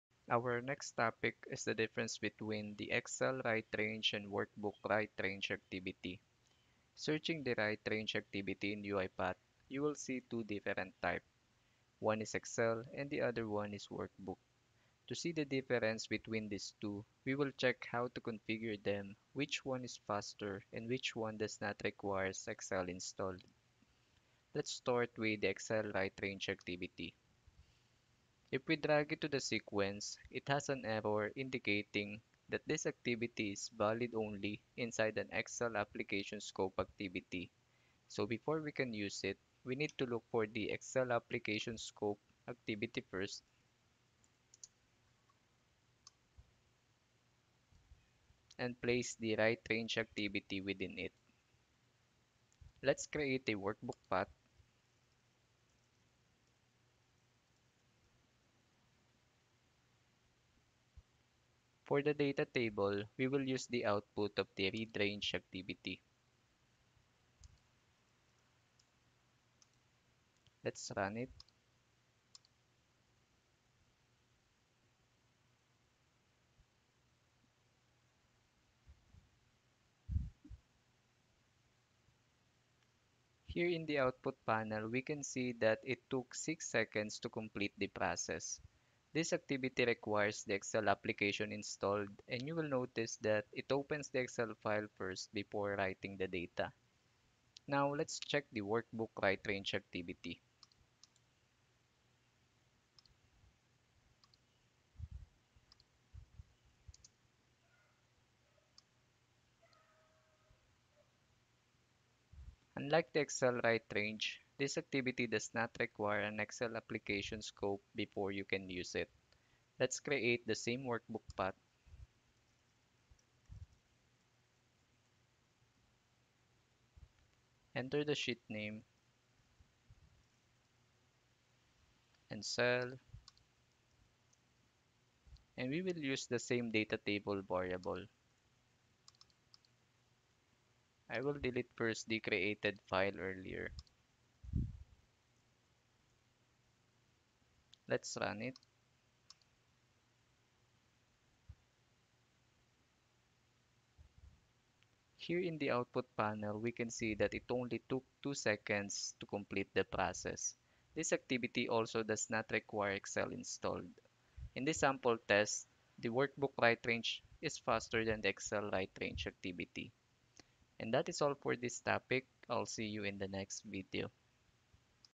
UiPath Write Range | Which one is faster?
UiPath Certification Review

In this video, we will review the UiPath Write Range activity. We will check which one is faster, the Excel Write Range or the Workbook Write Range. Let's also check which one requires Excel installed.